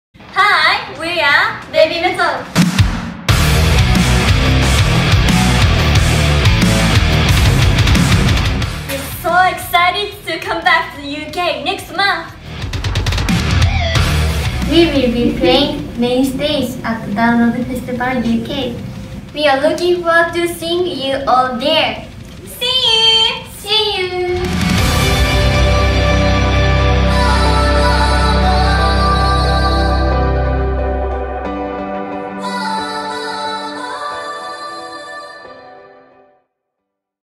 [SubEspañol] BABYMETAL anuncia su presentación en el festival Download 2016
Traducción Eng-Esp: Babymetal Fans México